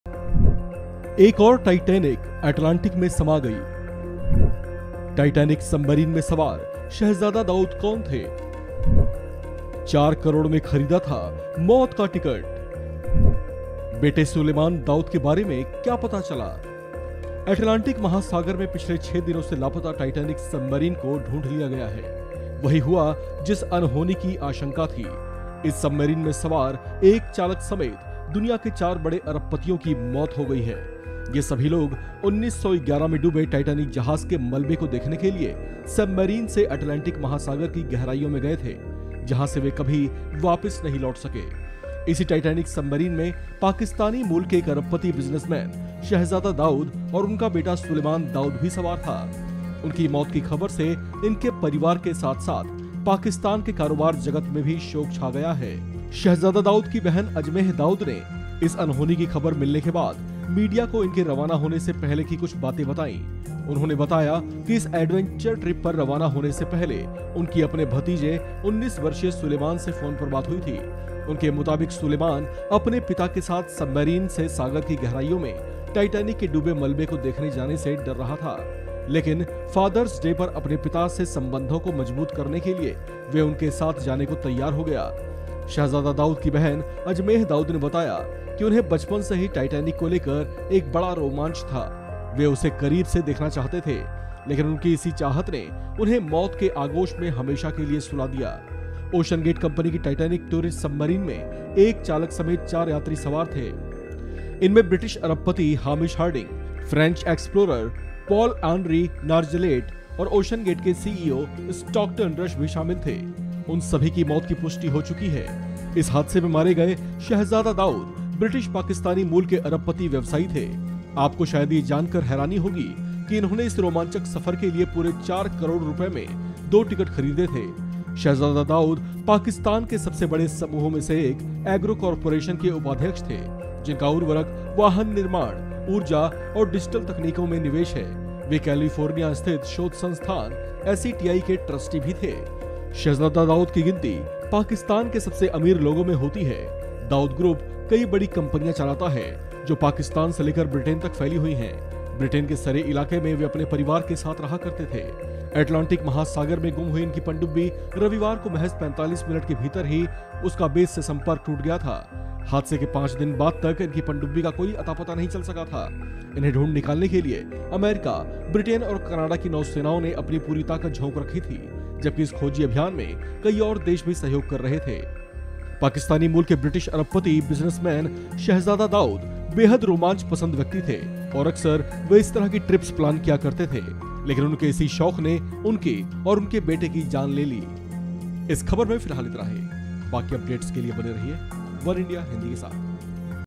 Titanic Missing Submarine में मारे गए Shahzada Dawood के बेटे की क्या जानकारी मिली | वनइंडिया हिंदी
Titanic Tourist Submarine Missing : अटलांटिक महासागर (Atlantic Ocean) में पिछले 6 दिनों से लापता टाइटैनिक सबमरीन (Titanic Submarine) को ढूंढ लिया गया है। वही हुआ जिस अनहोनी की आशंका थी। इस सबमरीन (Submarine) में सवार एक चालक समेत दुनिया के 4 बड़े अरबपतियों की मौत हो गई है। ये सभी लोग 1911 में डूबे टाईटैनिक जहाज़  (Titanic Ship) के मलबे को देखने के लिए सबमरीन से अटलांटिक महासागर की गहराइयों में गए थे। जहां से वे कभी वापिस नहीं लौट सके। इसी टाइटैनिक सबमरीन में पाकिस्तानी मूल के एक अरबपति बिज़नेसमैन शहज़ादा दाऊद (Shahzada Dawood) और उनका बेटा सूलेमान दाऊद (Suleman Dawood) भी सवार था। शहज़ादा दाऊद की बहन अजमेह दाऊद ने इस अनहोनी की खबर मिलने के बाद, मीडिया को इनके रवाना होने से पहले की कुछ बातें बताई। उन्होंने बताया कि, इस एडवेंचर ट्रिप पर रवाना होने से पहले उनकी अपने भतीजे 19 वर्षीय सूलेमान से फोन पर बात हुई थी। उनके मुताबिक सूलेमान अपने पिता के साथ सबमरीन से सागर की गहराइयों में टाइटैनिक के डूबे मलबे को देखने जाने से डर रहा था। ओशनगेट कंपनी की टाइटैनिक टूरिसट सबमरीन में 1 चालक समेत 4 यात्री सवार थे। इनमें (Hamish Harding), फ्रेंच एक्सप्लोरर पॉल आनरी नार्जेलेट (Paul Henri Nargelet) और ओशनगेट के CEO स्टॉकटन रश (Stockton Rush) भी शामिल थे।  (Tourist Submarine) (OceanGate) (Submersible Company)